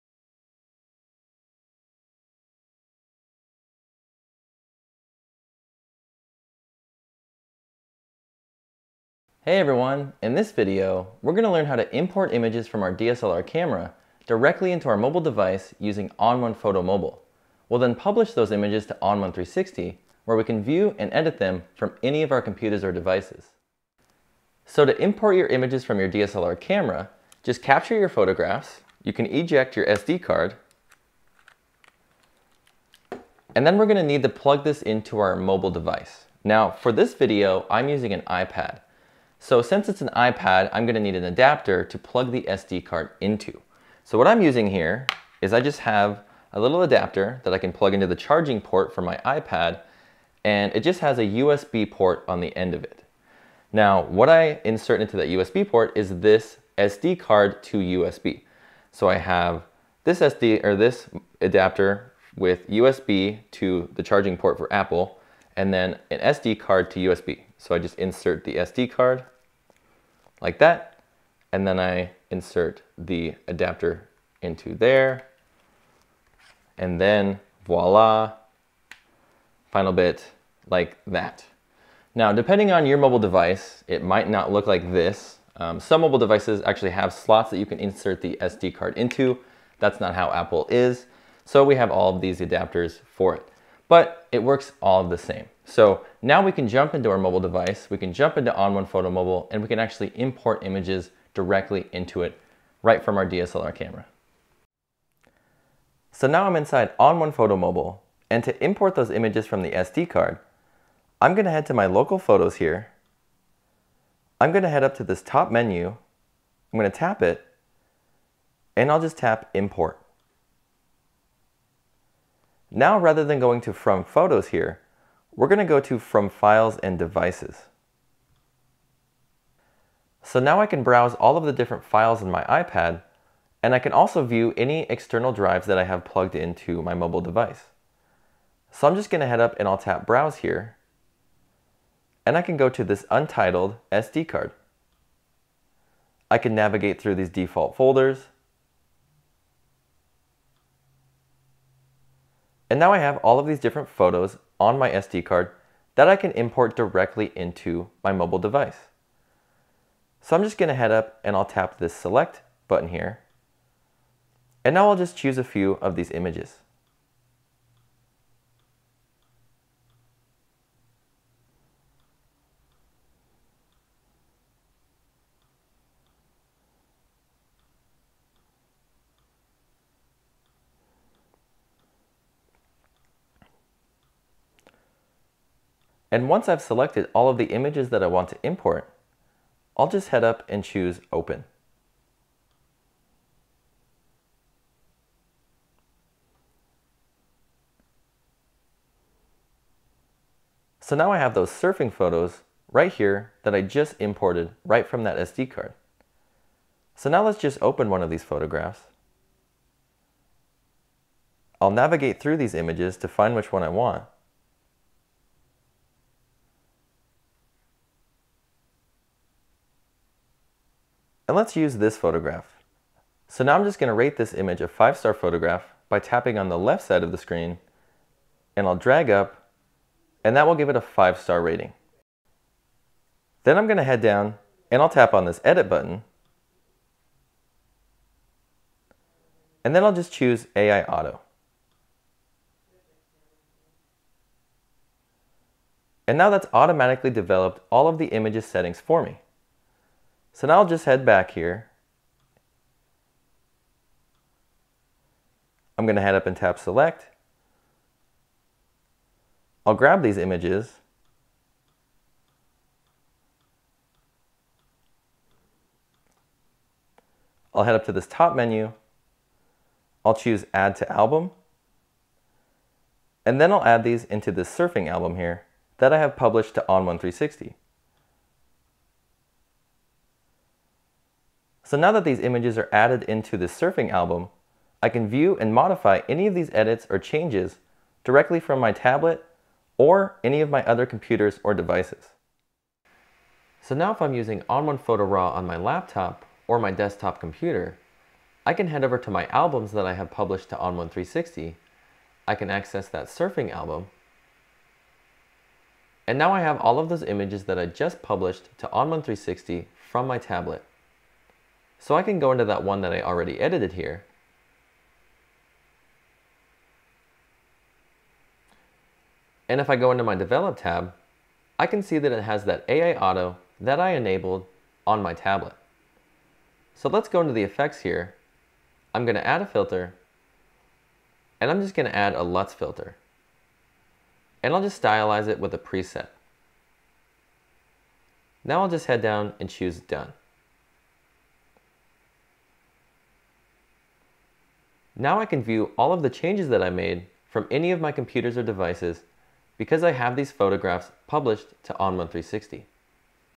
Importing Images into ON1 Photo RAW Mobile from Your Camera
Learn how to import images from your camera directly into your mobile device using ON1 Photo Mobile. From there we'll publish our images to ON1 360 so we can edit and adjust them from our laptop or desktop computers.